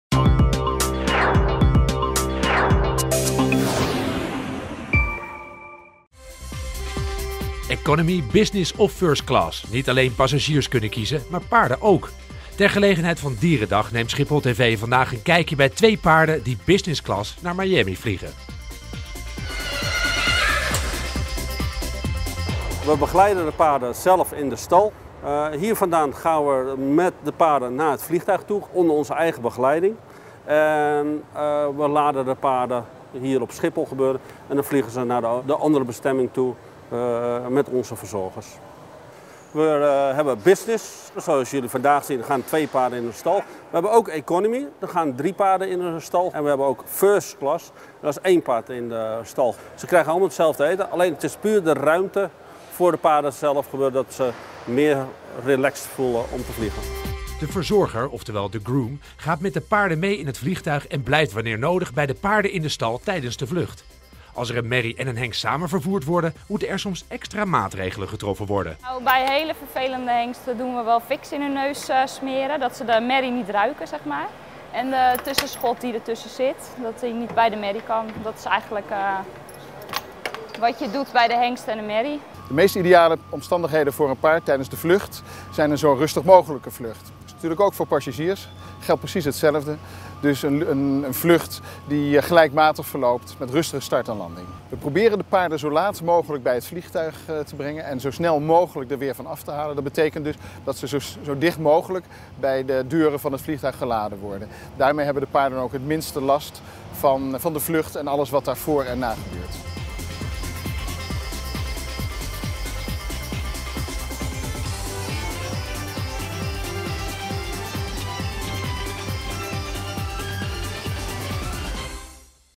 Paarden vliegen Business Class
Katten, honden en vogels. Op Schiphol worden dagelijks allerlei dieren vervoerd.  Speciaal voor dierendag volgen we 2 paarden die Business Class naar Miami vliegen.